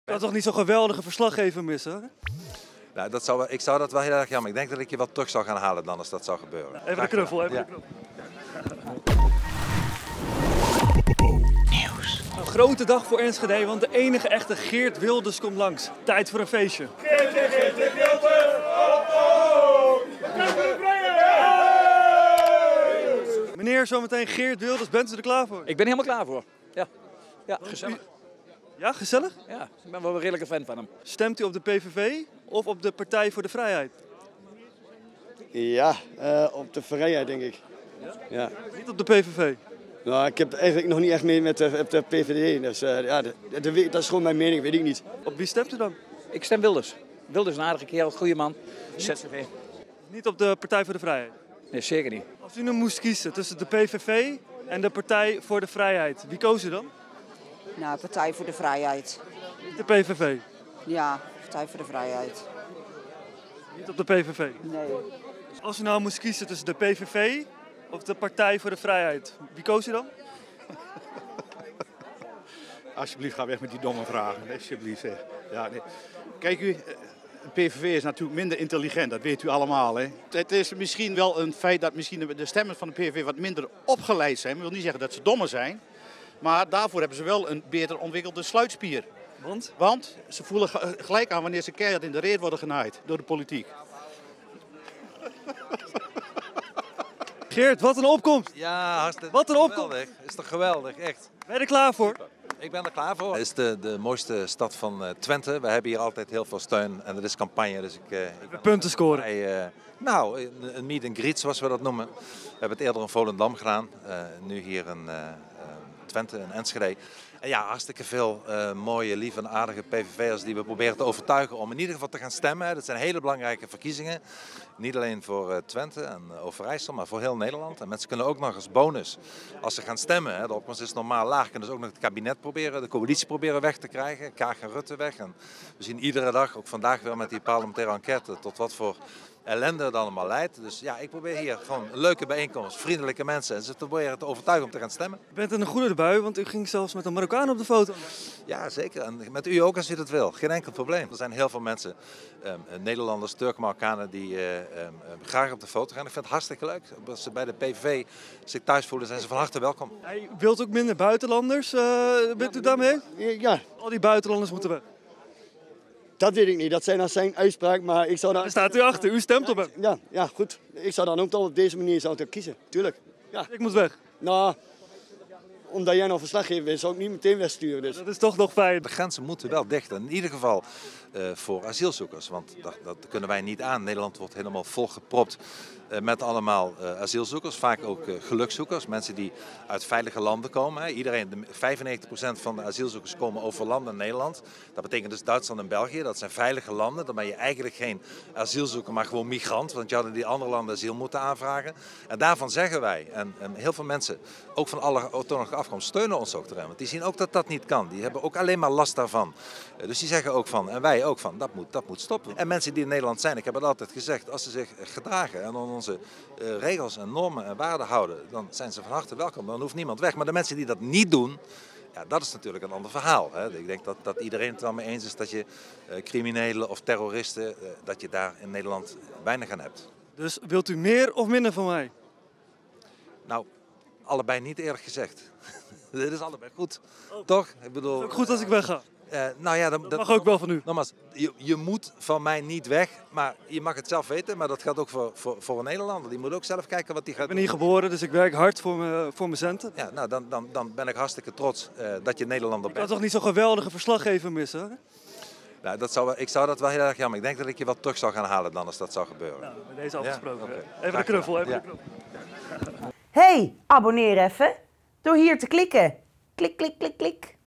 PVV populairder dan ooit: ‘Buitenlanders moeten weg’
Geert Wilders is populairder dan ooit en dus sturen wij onze exoot Aryan naar een PVV-partijbijeenkomst. Want wat zijn nou eigenlijk de sterkste punten van Geert?